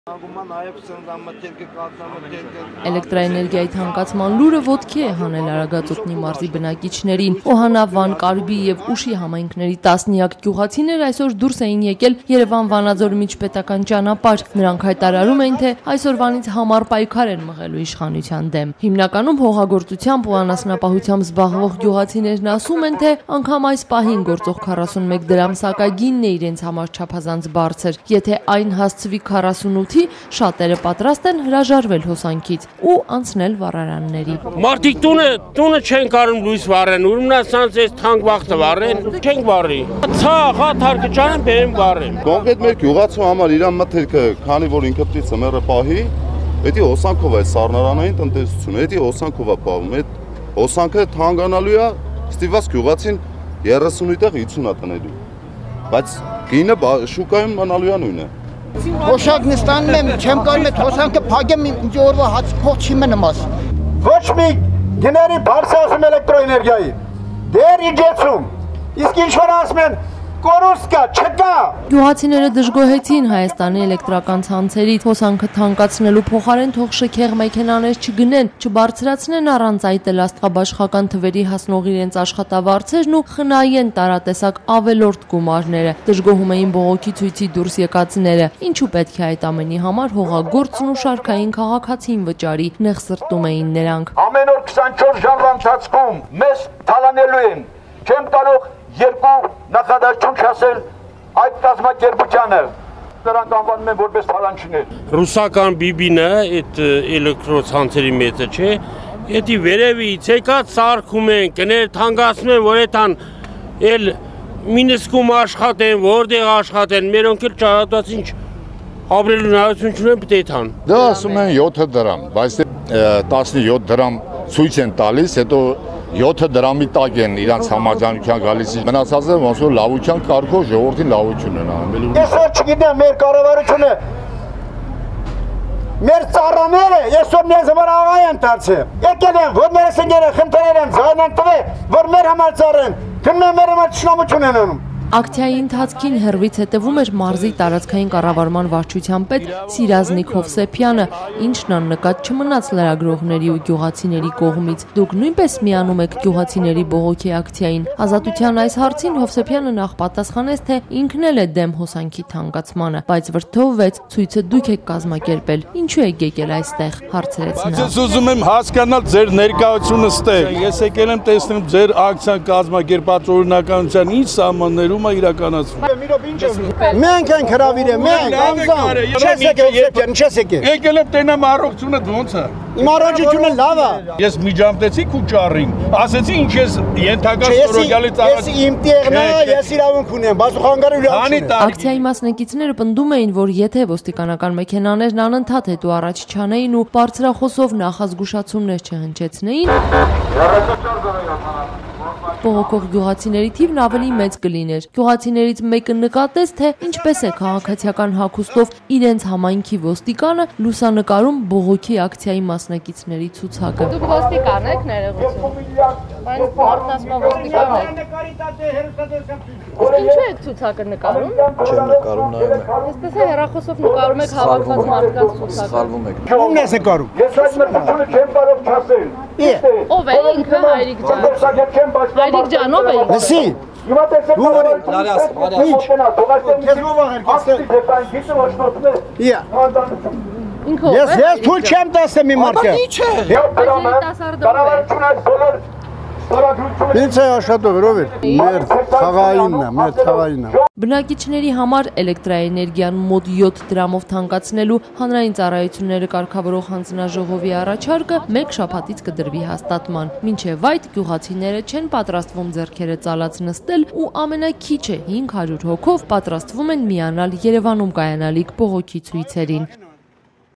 Հոսանքի թանկացման լուրը ոտքի է հանել Արագածոտնի մարզի բնակիչներին
Օհանավան, Կարբի և Ուշի համայնքների տասնյակ գյուղացիներ այսօր դուրս էին եկել Երևան-Վանաձոր միջպետական ճանապարհ: